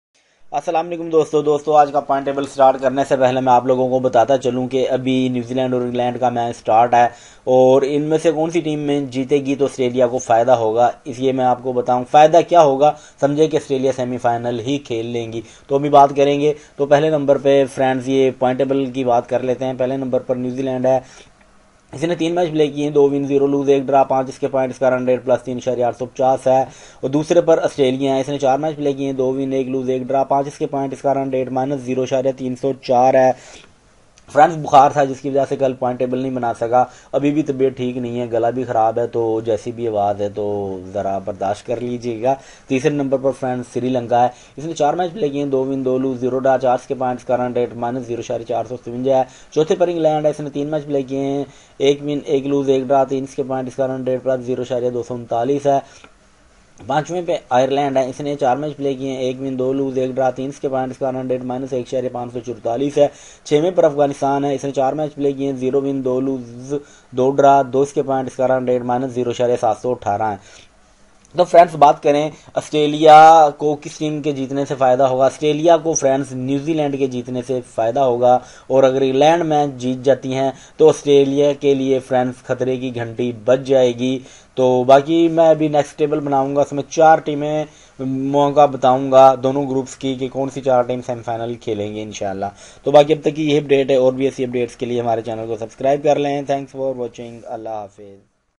icc t20 cricket world cup points table 2022 (Part 12 (01/11/2022) icc men's t20 world cup 2022
Icc T20 World Cup Points Table 2022 – The biggest competition among the cricketing nations teams is going to begin from May and cricket lovers are eagerly waiting for dismantling performances from their favorite players and teams. T20 World Cup 2022 points table will offer you the latest updates and news about teams points and rankings in the tournament. 16 teams are battling for the top position in the t20 world cup 2022 points table. India, New Zealand, Afghanistan, Pakistan, Australia, South
Africa, Bangladesh, England, SRI LANKA, NAMIBIA, NETHERLANDS, UNITED ARAB EMIRATES, ZIMBABWE, SCOTLAND, WEST INDIES, IRELAND,  and host Australia will participate in the tournament. This page will assist you to remain updated about the position of your favorite team in the T20 cricket world cup. On the other side, you can also analyse outcome of upcoming contests on the basis of the current form and ranking of any team.
The competitive format will ensure the tough contest among teams as they will try hard to book their place for the next round.
Two points will be given for each win and for tie, one point will be given to both teams. In case of no result due to rain or any other factor, both teams will still get one point. At the end of T20 World Cup 2022 group stage, top four teams with higher points will make their place in the semi-finals. T20 World Cup 2022 Points Table will be updated as soon as the match ends so you’ll always get updated about the recent teams’ rankings.
Standings are updated with the completion of each game
M:
The number of matches played.
W:
The number of matches won.
L:
The number of matches lost.
T:
The number of matches tied.
N/R:
The number of matches abandoned.
PT:
Number of points awarded.
NRR:
Net Run Rate.